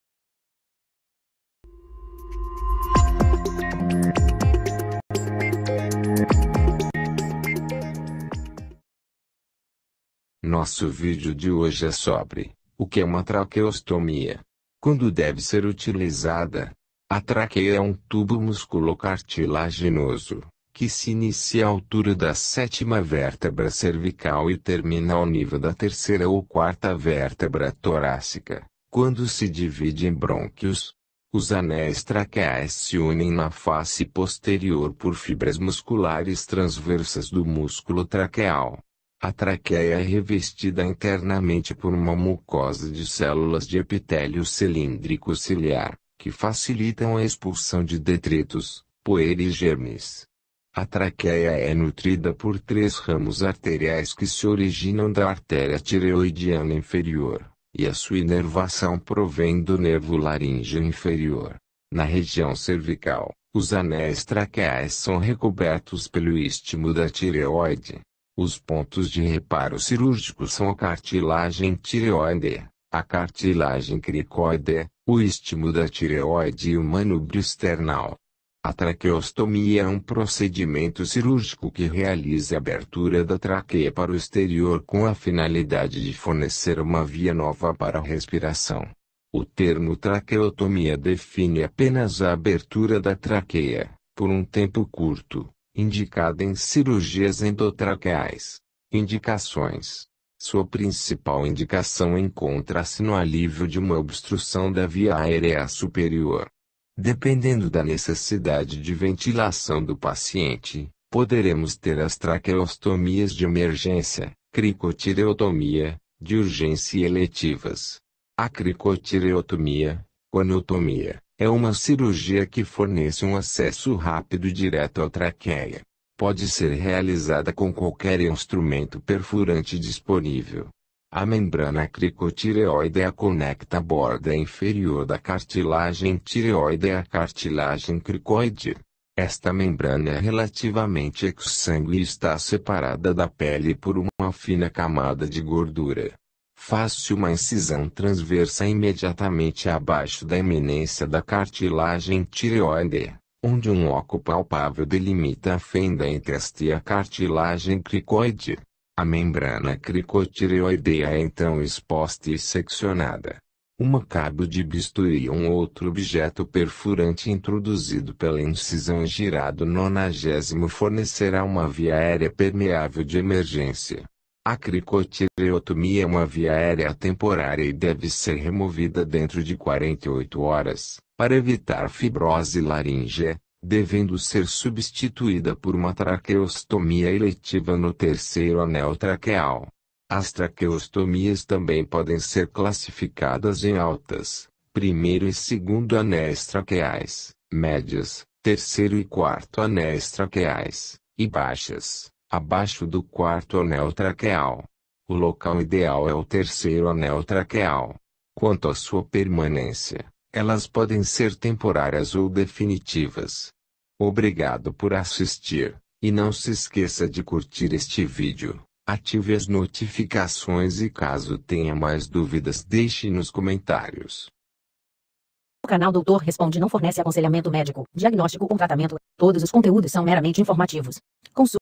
O que é uma Traqueostomia? Quando deve ser utilizada? - Doutor Responde
Doutor Responde

Todos os dias uma dica para melhorar sua saúde.

A traqueia é um tubo musculocartilaginoso, que se inicia à altura da sétima vértebra cervical e termina ao nível da terceira ou quarta vértebra torácica, quando se divide em brônquios. Os anéis traqueais se unem na face posterior por fibras musculares transversas do músculo traqueal. A traquéia é revestida internamente por uma mucosa de células de epitélio cilíndrico ciliar, que facilitam a expulsão de detritos, poeira e germes.  A traquéia é nutrida por três ramos arteriais que se originam da artéria tireoidiana inferior, e a sua inervação provém do nervo laríngeo inferior.  Na região cervical, os anéis traqueais são recobertos pelo istmo da tireóide.  Os pontos de reparo cirúrgico são a cartilagem tireóidea, a cartilagem cricóidea, o istmo da tireóide e o manúbrio esternal.  A traqueostomia é um procedimento cirúrgico que realiza a abertura da traqueia para o exterior com a finalidade de fornecer uma via nova para a respiração. O termo traqueotomia define apenas a abertura da traqueia, por um tempo curto, indicada em cirurgias endotraqueais.  Indicações Sua principal indicação encontra-se no alívio de uma obstrução da via áerea superior. Dependendo da necessidade de ventilação do paciente, poderemos ter as traqueostomias de emergência (cricotireotomia), de urgência e eletivas. A cricotireotomia (coniotomia) é uma cirurgia que fornece um acesso rápido e direto à traquéia. Pode ser realizada com qualquer instrumento perfurante disponível. A membrana cricotireóidea conecta a borda inferior da cartilagem tireóidea à cartilagem cricóidea. Esta membrana é relativamente exsangüe e está separada da pele por uma fina camada de gordura. Faz-se uma incisão transversa imediatamente abaixo da iminência da cartilagem tireóidea, onde um oco palpável delimita a fenda entre esta e a cartilagem cricóidea. A membrana cricotireoidea é então exposta e seccionada. Um cabo de bisturi ou um outro objeto perfurante introduzido pela incisão e girado 90º fornecerá uma via aérea permeável de emergência.  A cricotireotomia é uma via aérea temporária e deve ser removida dentro de 48 horas, para evitar fibrose laríngea, devendo ser substituída por uma traqueostomia eletiva no terceiro anel traqueal.  As traqueostomias também podem ser classificadas em altas (primeiro e segundo anéis traqueais), médias (terceiro e quarto anéis traqueais) e baixas (abaixo do quarto anel traqueal). O local ideal é o terceiro anel traqueal. Quanto à sua permanência, elas podem ser temporárias ou definitivas.